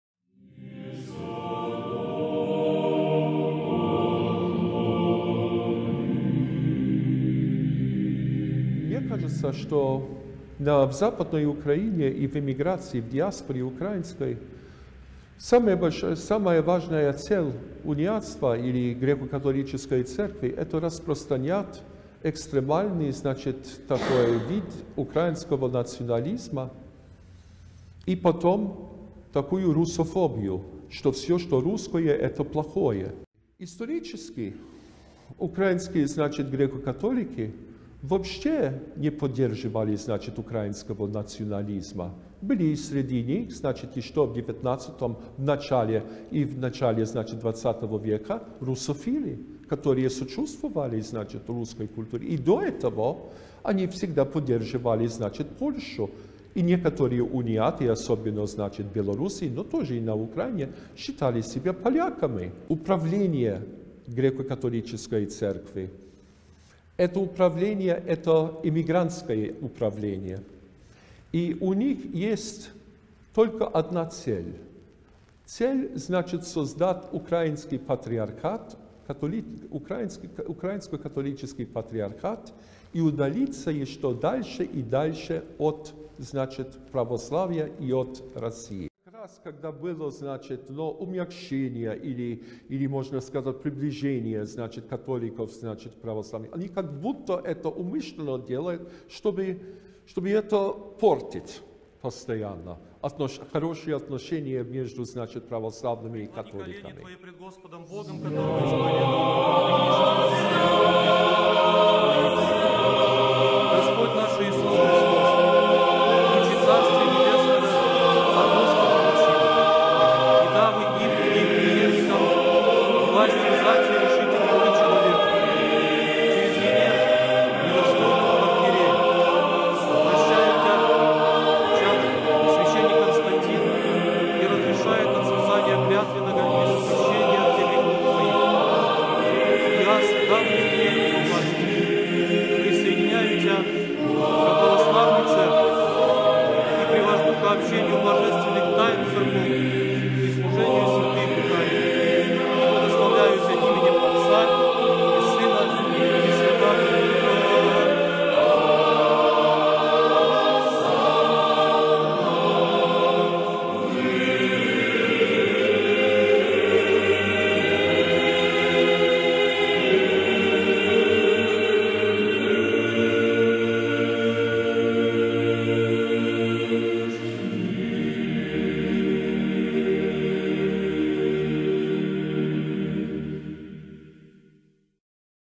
Ex Jesuit exposes evil role of Greek-Catholic Uniates in Ukraine
Father Constantine Simon, Orthodox monk-priest who converted from Roman-Catholicism to the Orthodox Catholic Faith in 2014 was Vatican's jesuit adviser and professor of Russian Orthodox history and language.